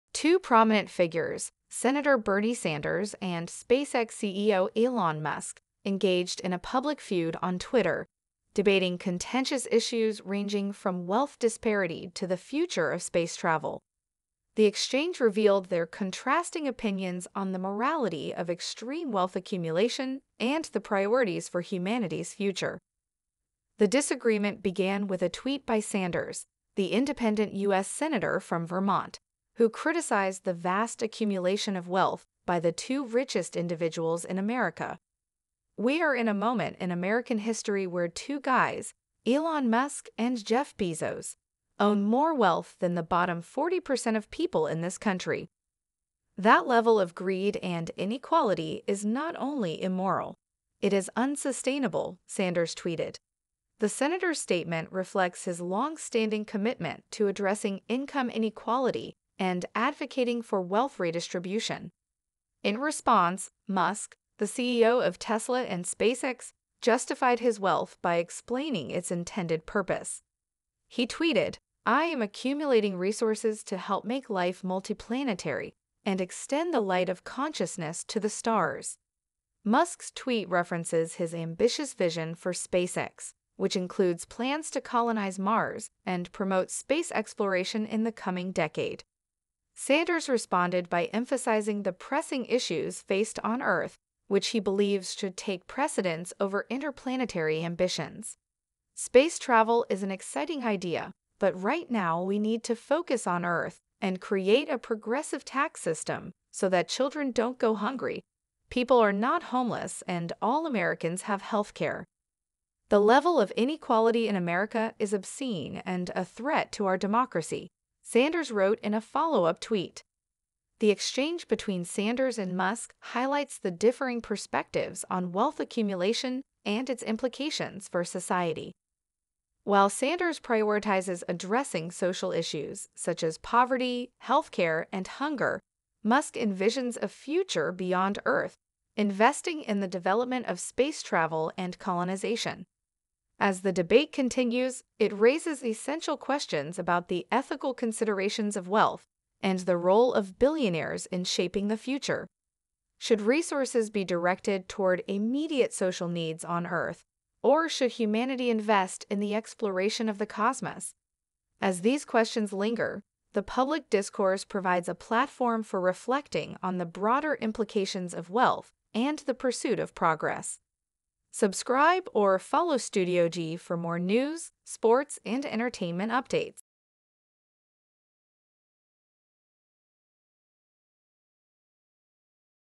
Bernie Sanders And Elon Musk Public Feud - What's Next For The Rich
Two prominent figures, Senator Bernie Sanders and SpaceX CEO Elon Musk, engaged in a public feud on Twitter, debating contentious issues ranging from wealth disparity to the future of space travel. The exchange revealed their contrasting opinions on the morality of extreme wealth accumulation and the priorities for humanity's future.